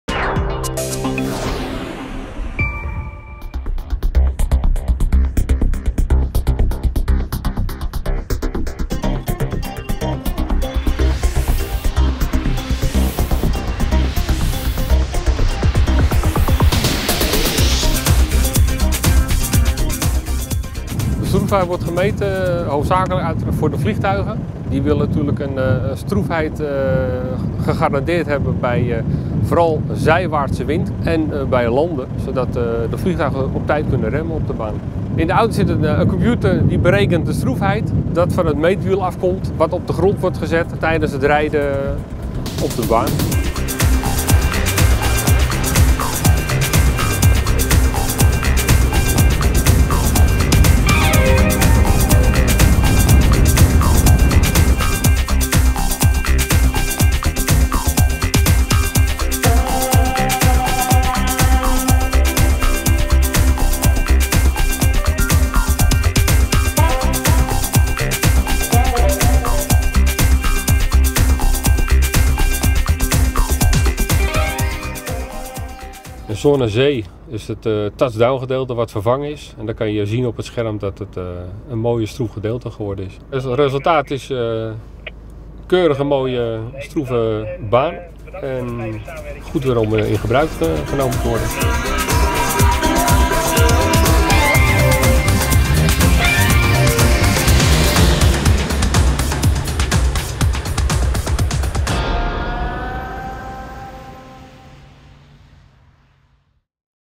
Polderbaan weer open na stroefheidsmeting
Na een wekenlange onderhoudsbeurt is de Polderbaan op zondag 20 april weer in gebruik genomen. Eén van de laatste werkzaamheden op de Polderbaan was een zogenaamde stroefheidsmeting.